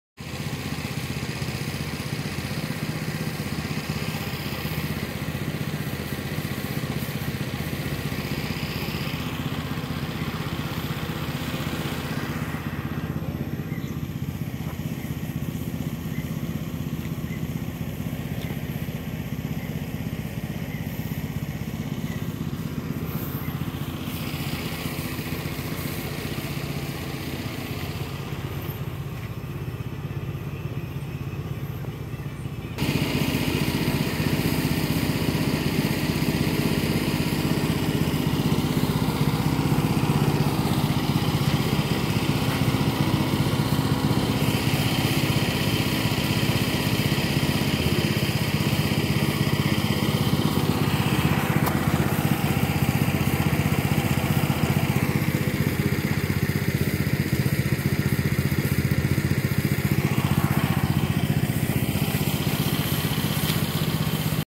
akibat belum ada jembatan nyebrang sunggai kecil aja nunggu berjam_jam
akibat belum ada jembatan nyebrang sunggai kecil aja nunggu sampai berjam_jam, di harapkan kepada pemerintah kab sanggau agar segera di bangunkan jembatan di kec kapuas kab sanggau kalbar,agar masyarakat di sekitar di permudah aksesnya, dan untuk memajukan ekonomi masyarakat sekitar, jangan uangnya habis masuk ke dalam kocek.